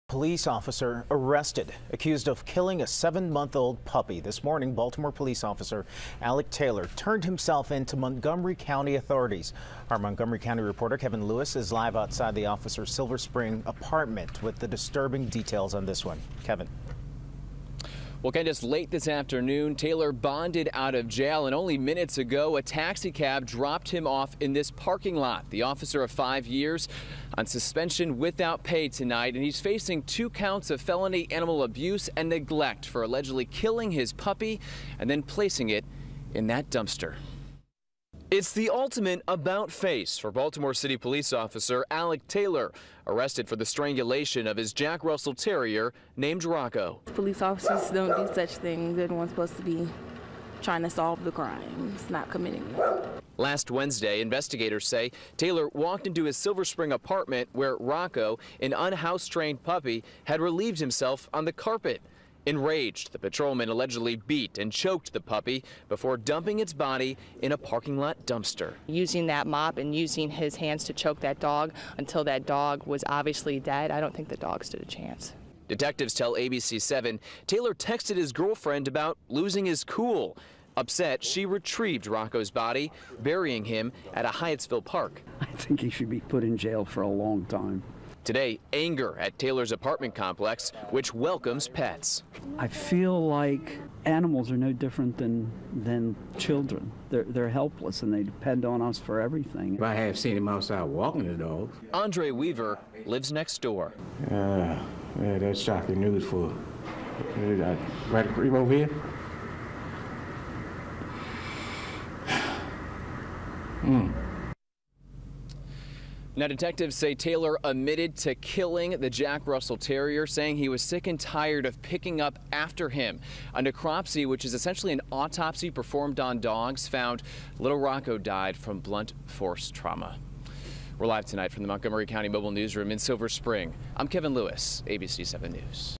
Cop Strangles Girlfriends Puppy, Then Sends Pics Of Dead Puppy To Girlfriend
A very typical cop, doing what any cop would do given the same circumstances. Any woman that goes onto a relationship with a cop is playing a losing game.